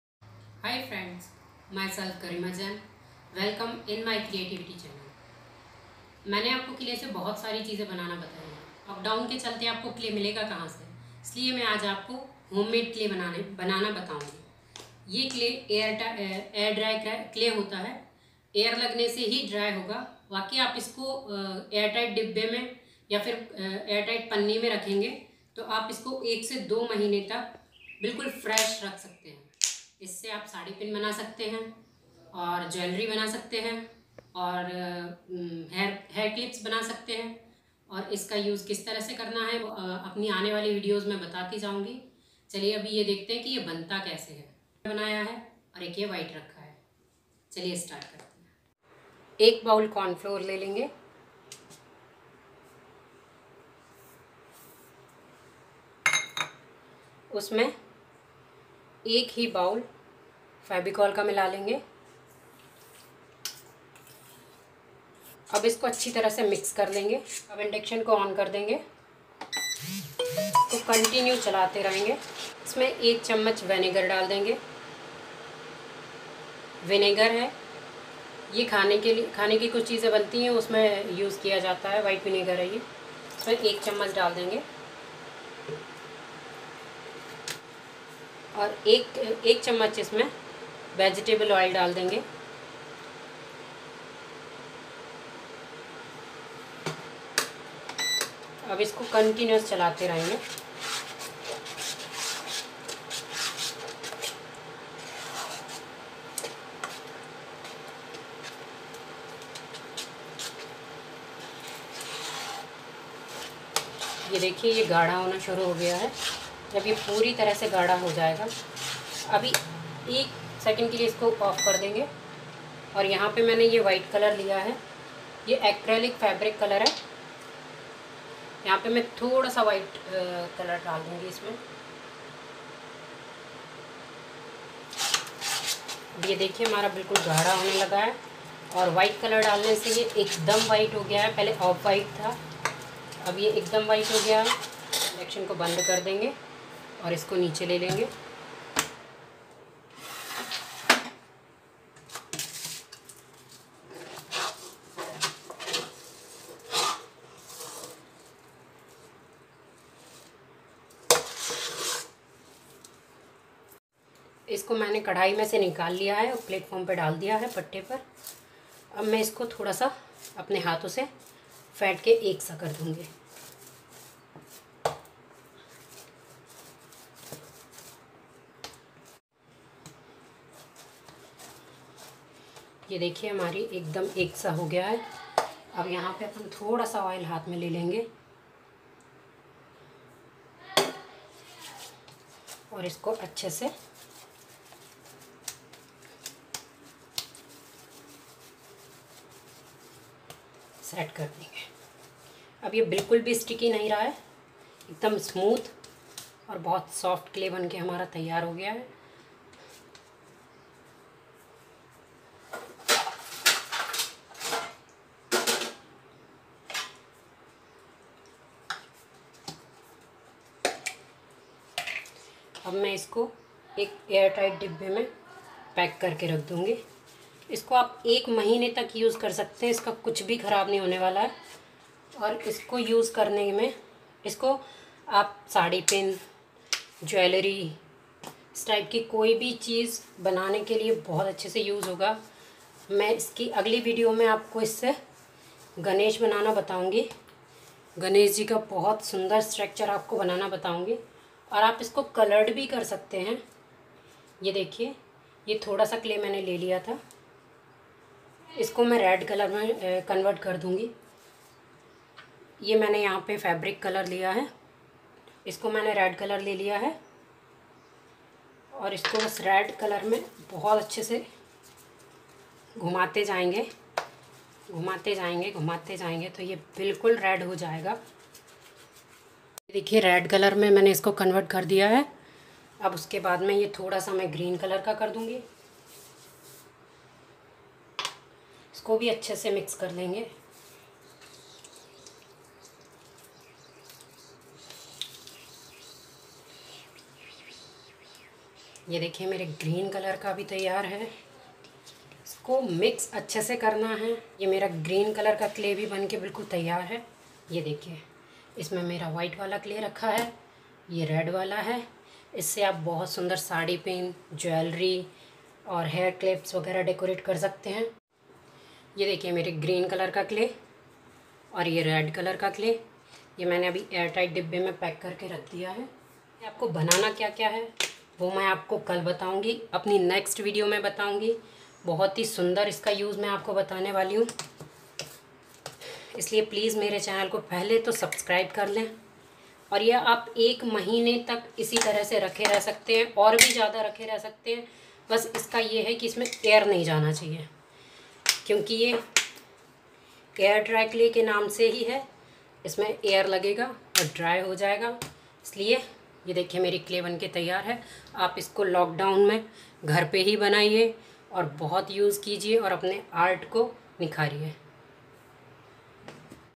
Home made air dry clay
Is clay ka use aap saree pin hair clips decoration A1 jewellery banane ke liye kar sakte hain